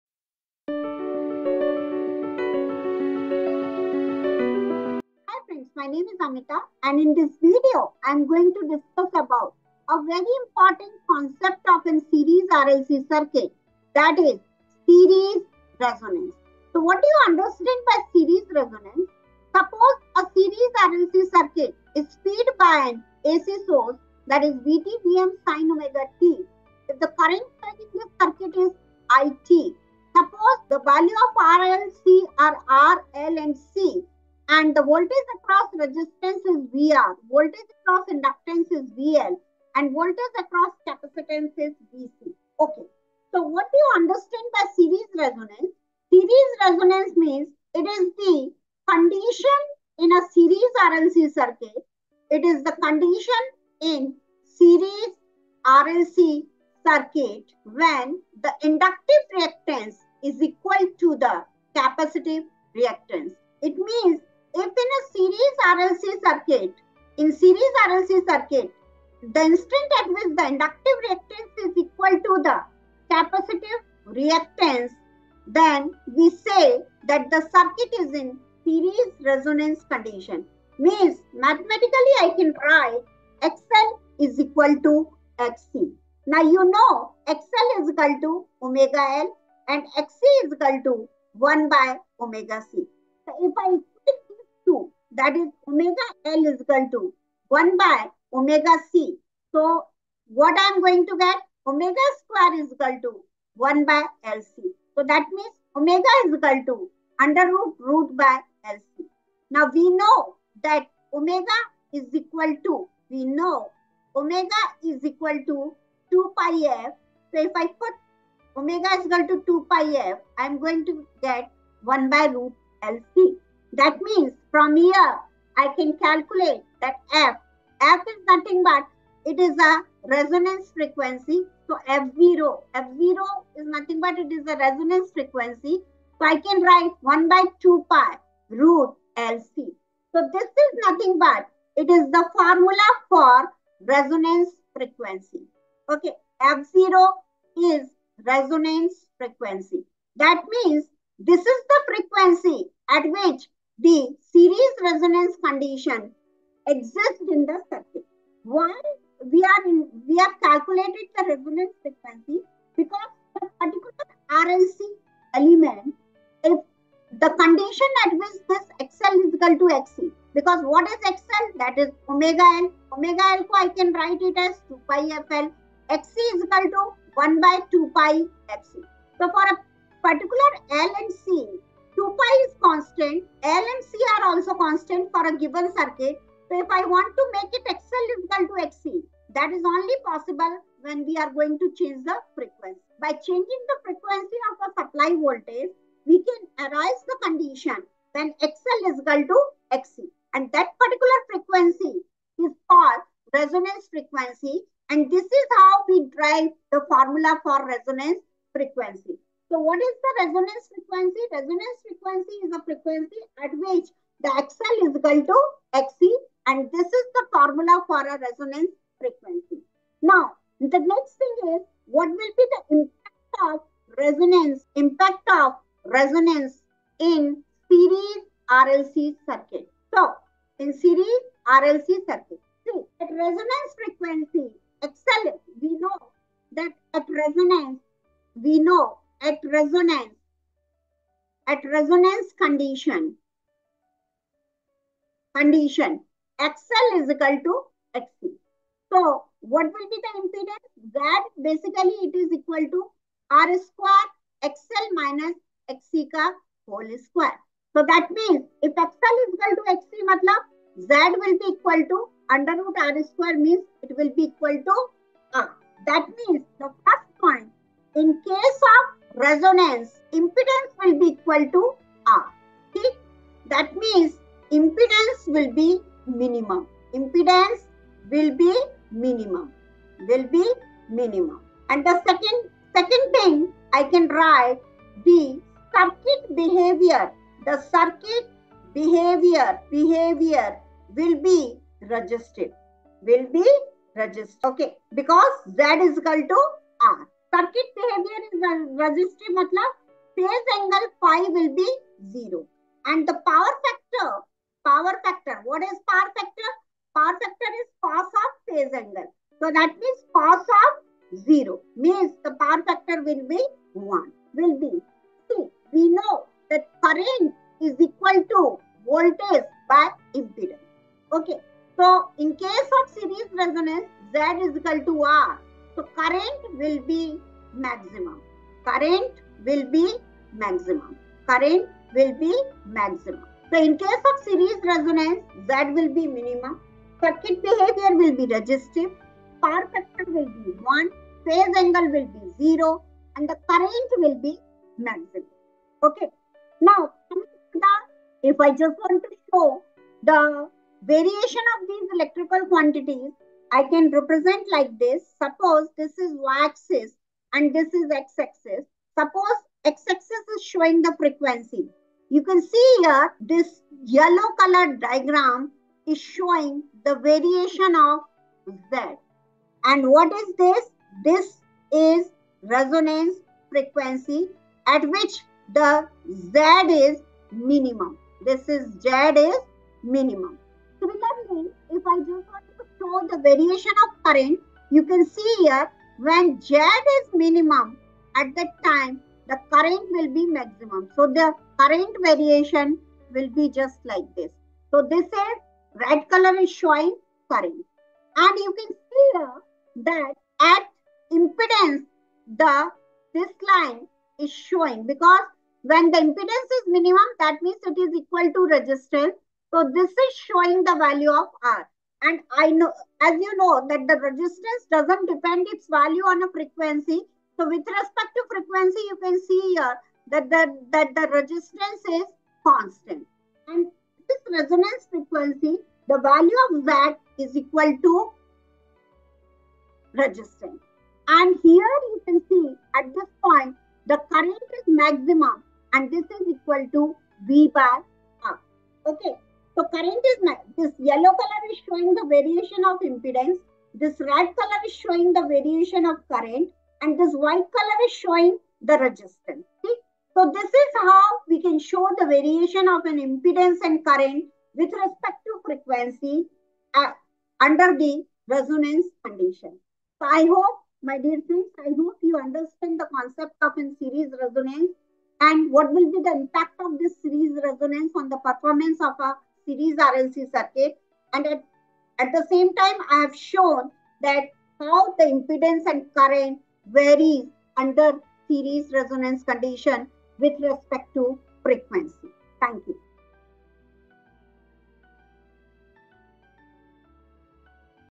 series resonance
In this video the concept of series resonance and its impact on circuit are explained.